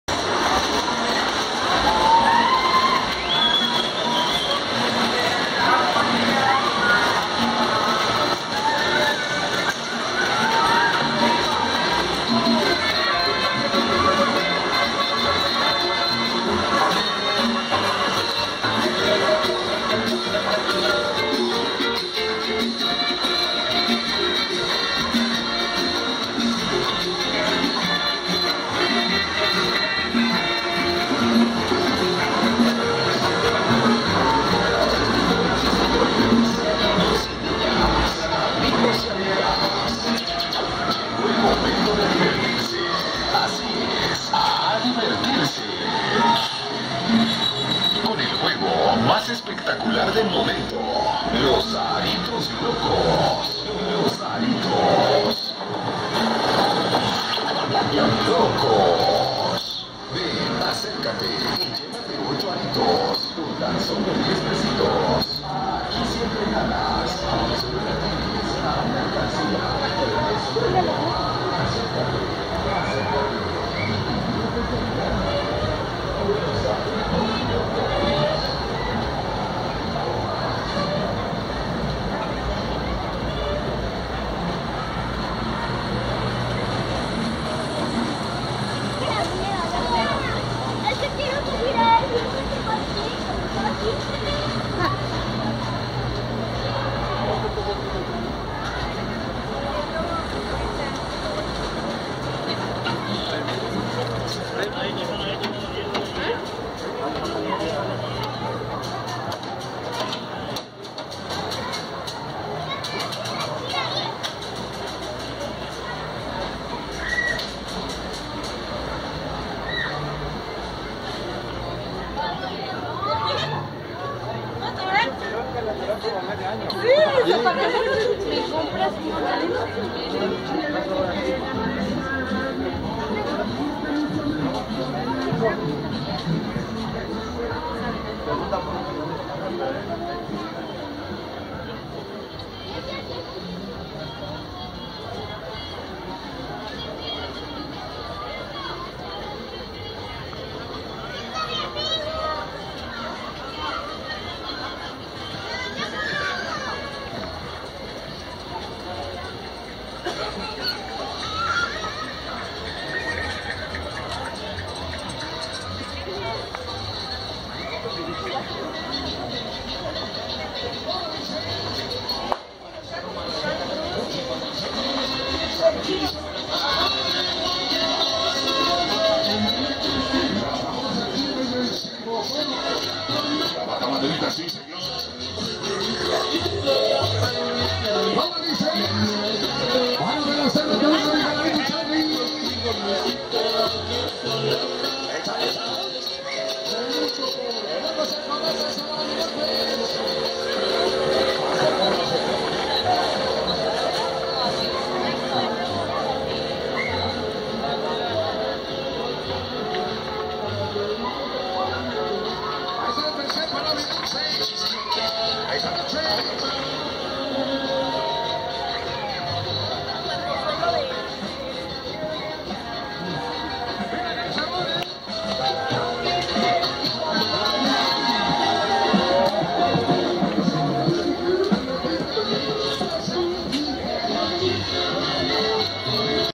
ZINACANTEPEC - CAMINANDO UNA FERIA - PARTE 1 - Domingo, 3 de Diciembre 2023, Zinacantepec, Edo Mex
ZINACANTEPEC - CAMINANDO UNA FERIA - PARTE 1 - Domingo, 3 de Diciembre 2023, Zinacantepec, Estado de Mexico, Mexico